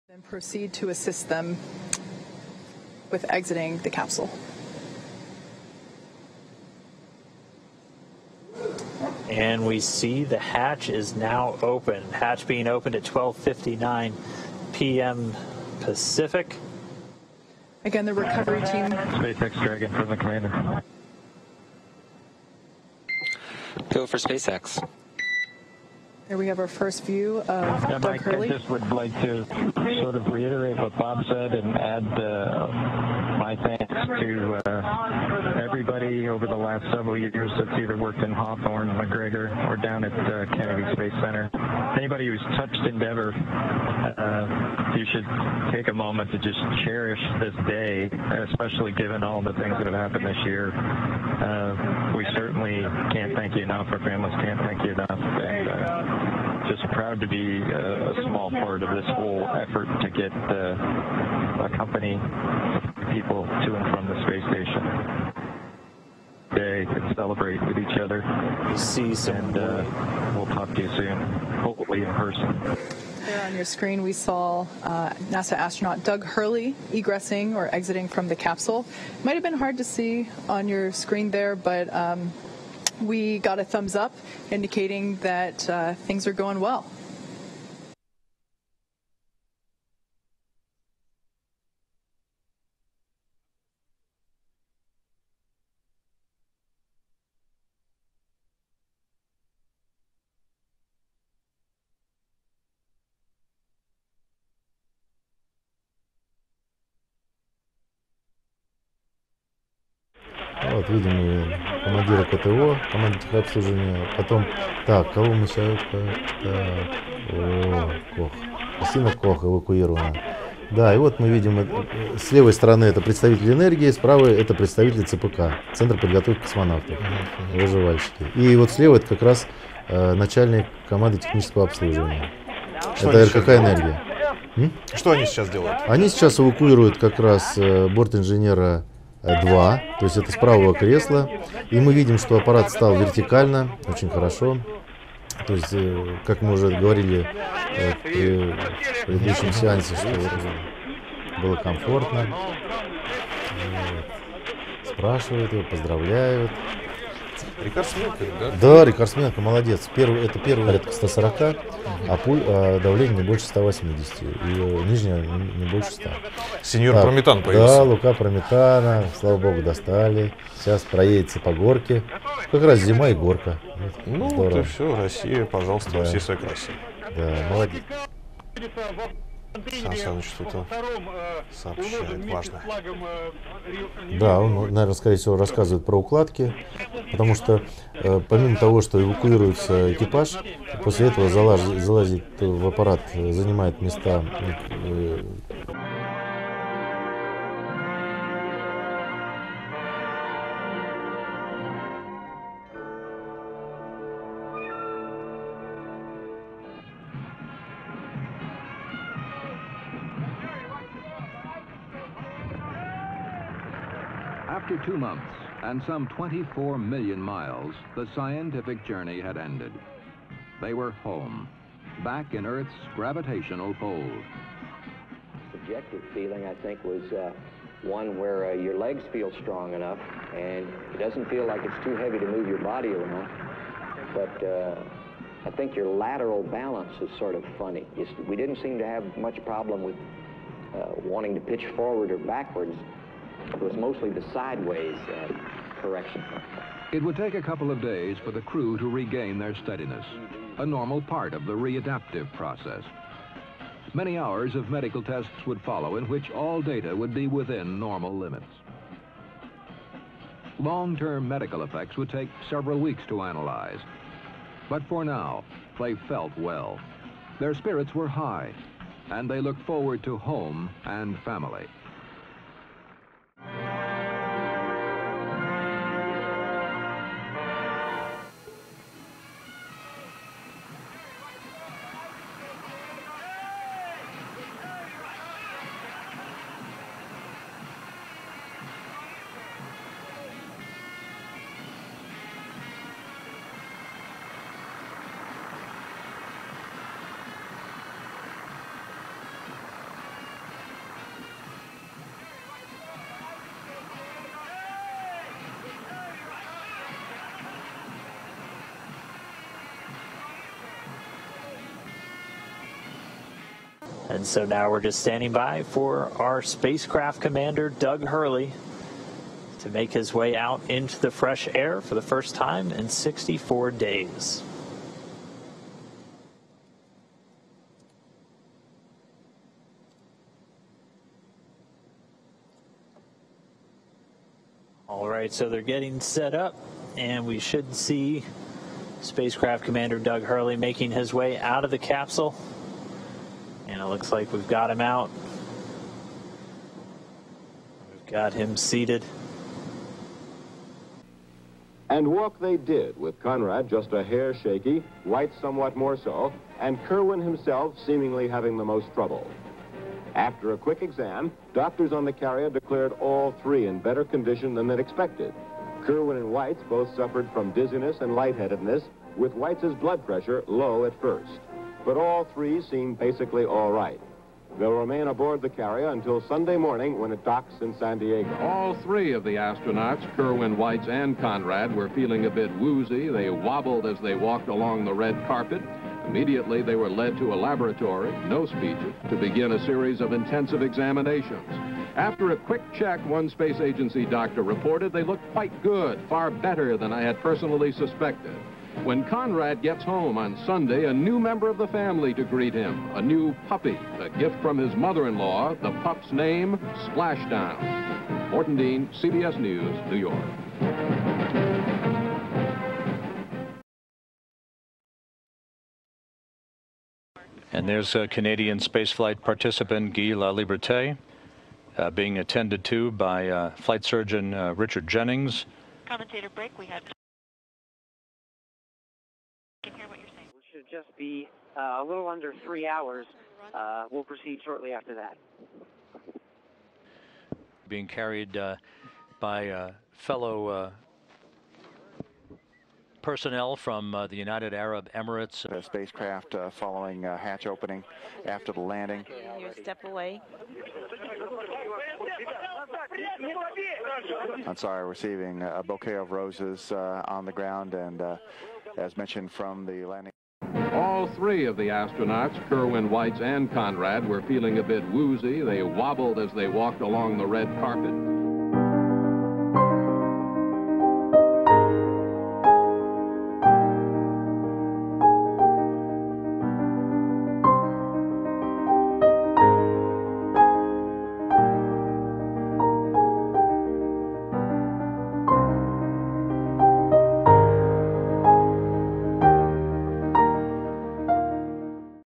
Crew Dragon & SkyLab
The August 2nd 2020 Crew Dragon splashdown has even more historical significance than we think.
This is the very first time when American astronauts went into space in their own national space capsule!

“The third crew (Skylab 4) returned in better physical condition than the other two crews who had spent less time in space.”